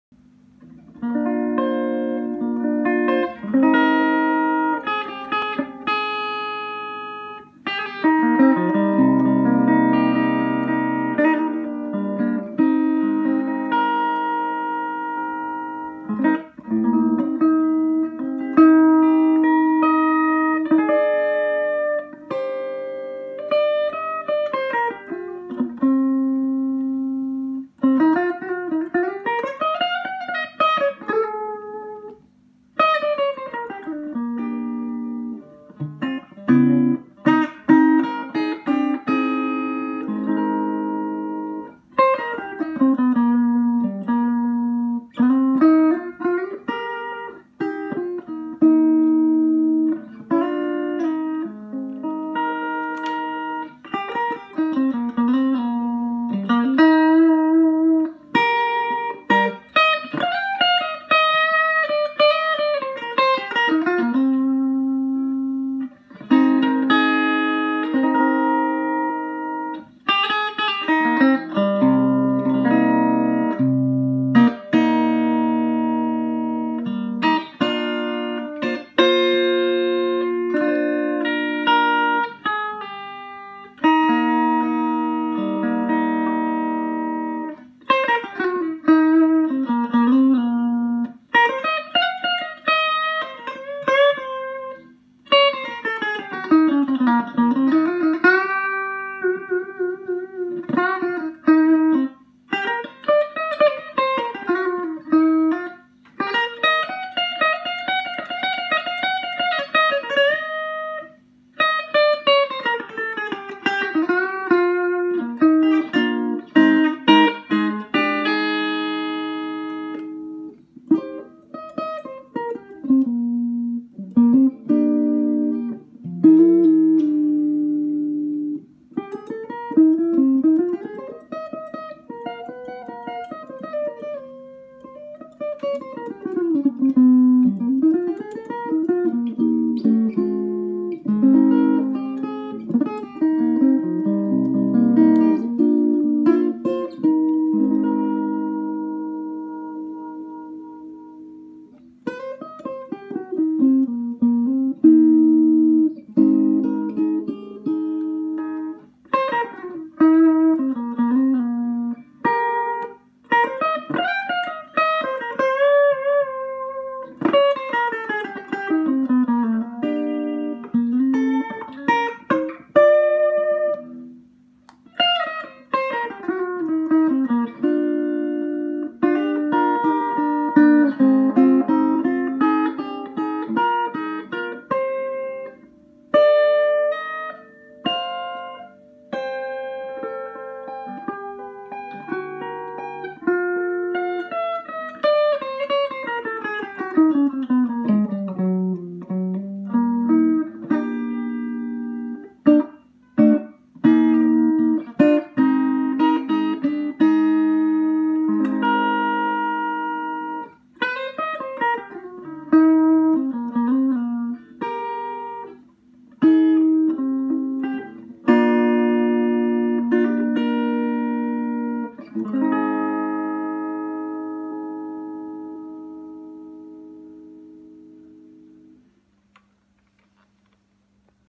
Try Jazz on Fender Esquire, with Fender 57 Custom Champ Jazzy licks impros demo
When we plugged in Champ, every gear goes Jazzy imo.
I played on bridge position, which is bypassed tone control, I could Jazz.
Little trebly, so I went bluey on latter half but Jazzy enough!